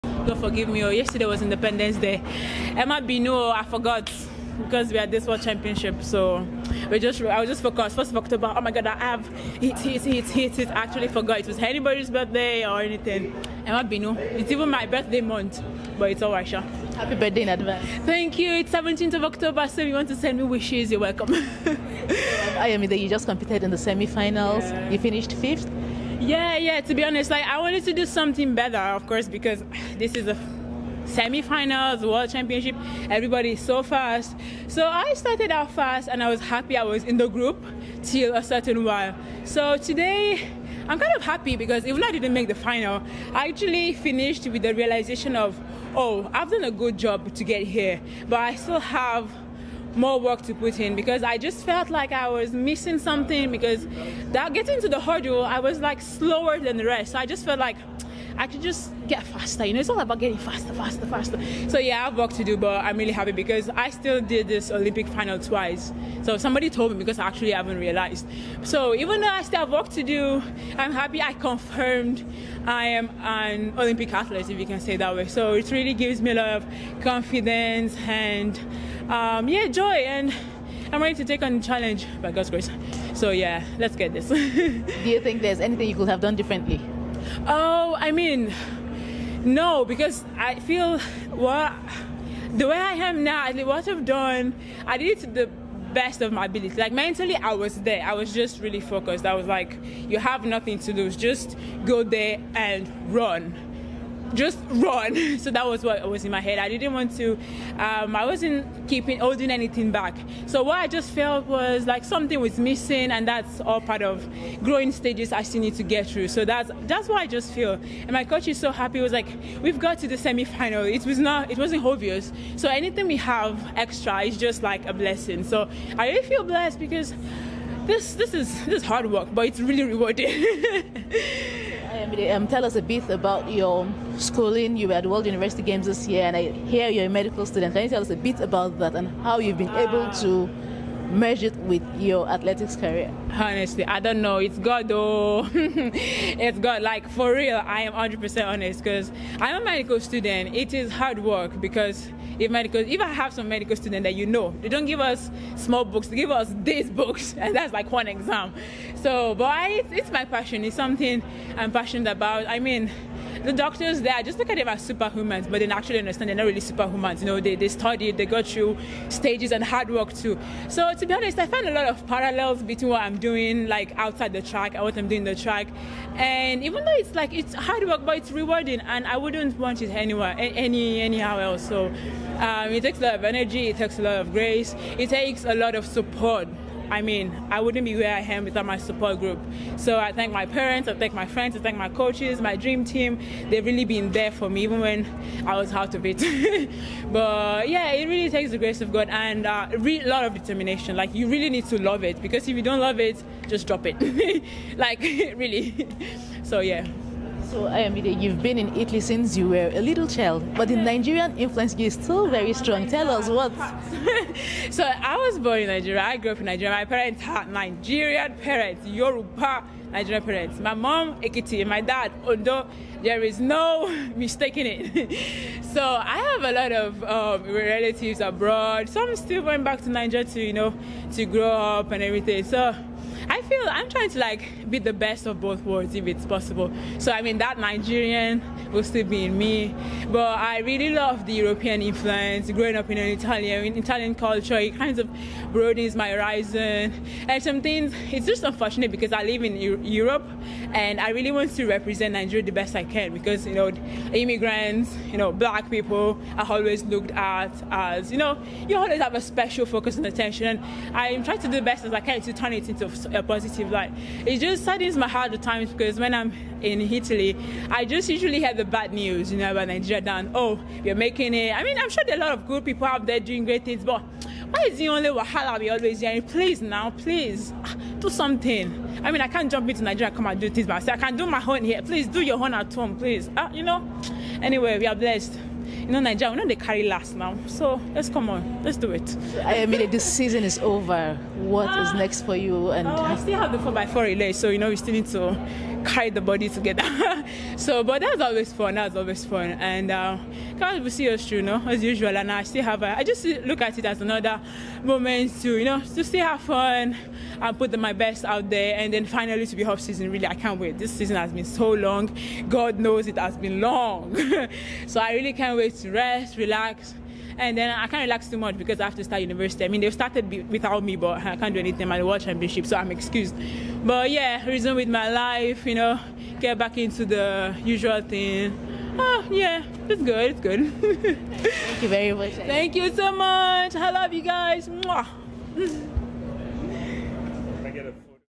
Ayomide FOLORUNSO ITA Doha 2019 IAAF World Championships Women's 400m Hurdles Semis
Ayomide Folorunso is satisfied with her performance at the 2019 IAAF World Championships in Doha where she made the semis of the women's 400m Hurdles, and she sends her belated Independence Day wishes to her Nigerian fans.